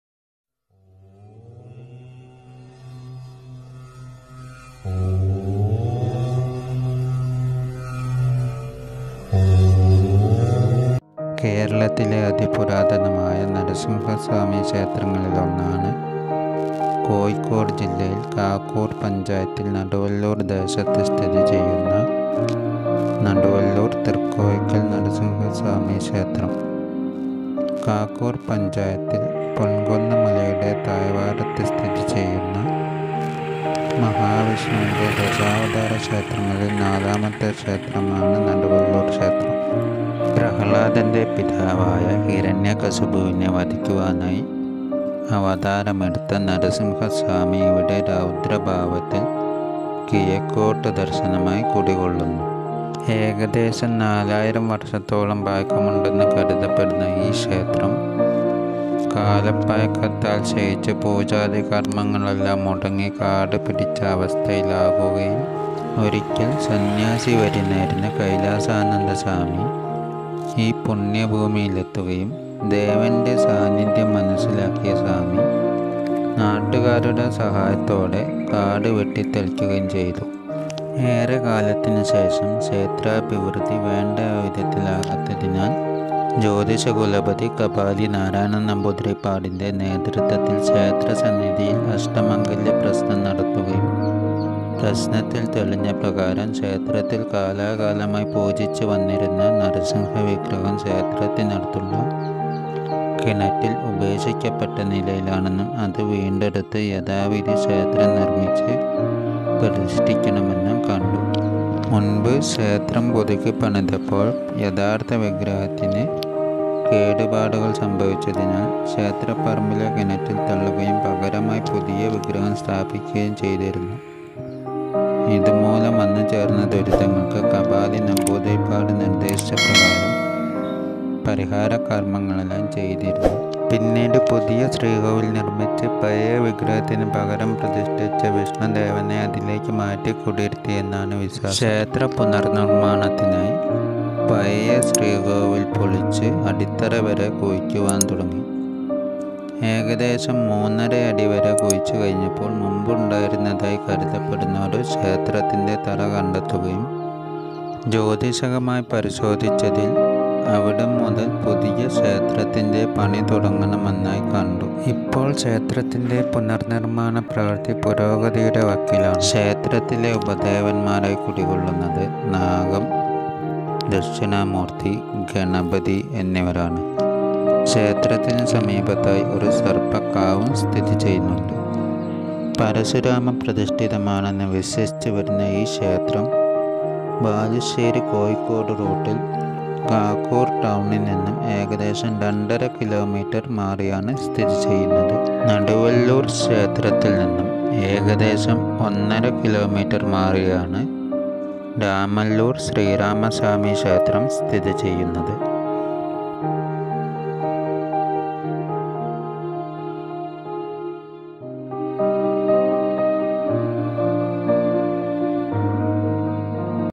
നടുവല്ലൂർ ശ്രീ നരസിംഹമൂർത്തി ക്ഷേത്രം
ദശവതാര ക്ഷേത്രങ്ങളിൽ നാലാമത്തെ ക്ഷേത്രം

ഹിരണ്യകശുപുവിനെ വധിച്ച ശേഷമുള്ള രൗദ്ര ഭാവം

കോഴിക്കോട് ബാലുശ്ശേരി റൂട്ടിൽ കാക്കൂർ ടൗണിൽ നിന്നും ഏകദേശം രണ്ടര കിലോമീറ്റർ മാറിയാണ് ക്ഷേത്രം ഉള്ളത്